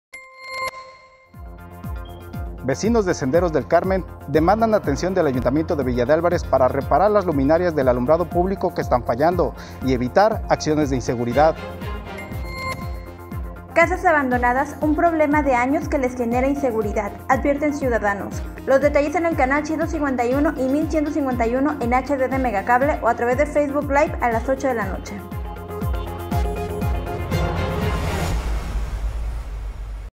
AvanceInformativo | 🗣️ 💡🏘️🚨+ Vecinos de Senderos del Carmen demandan la reparación de luminarias
+ Vecinos de Senderos del Carmen demandan la reparación de luminarias

+Casas abandonadas un problema de años que genera inseguridad, advierten ciudadanos